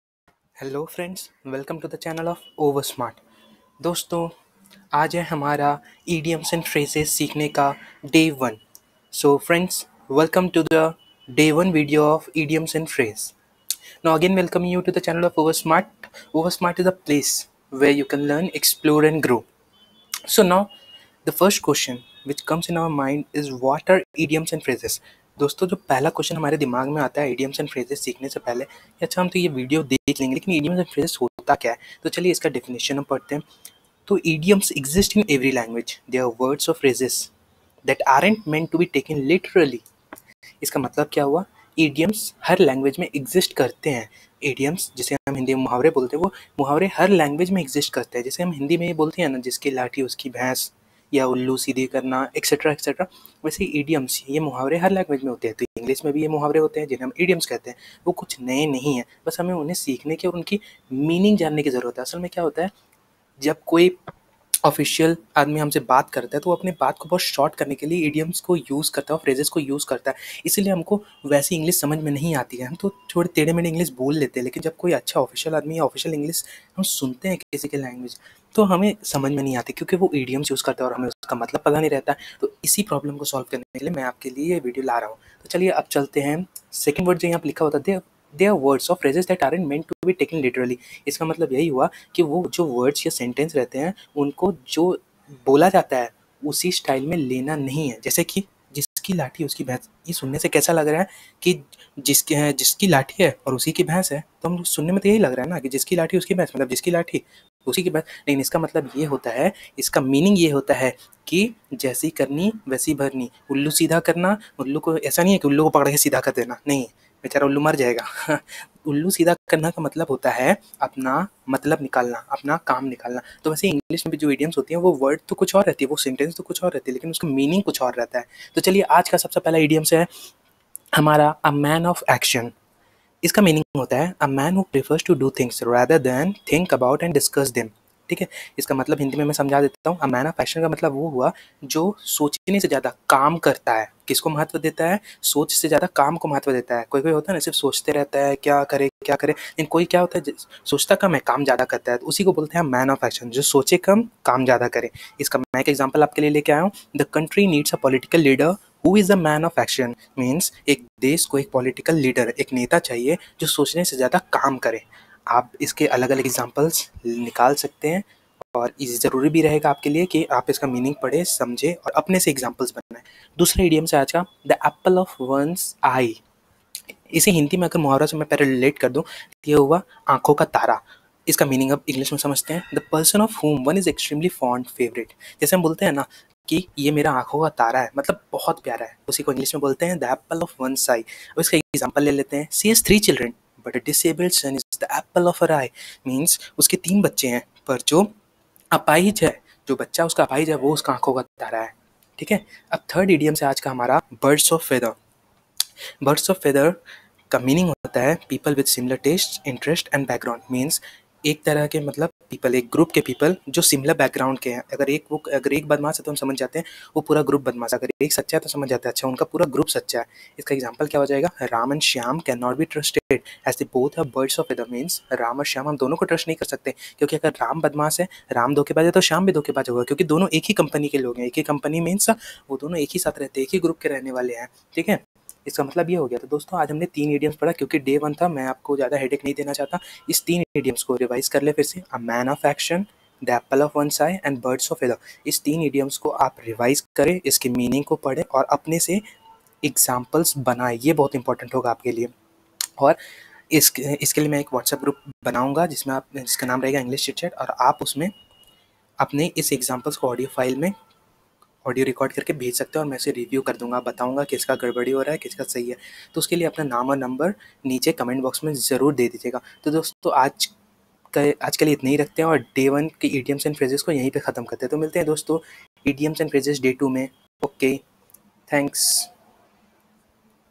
idioms and phrases day 1 @ oversmart
Dear friends this is a new innitative from oversmart to teach students and people who fear of english language.In this session we will teach idioms and phrases of english and there application.

There will also be a whatsapp group for help and support and likewise for self assessment.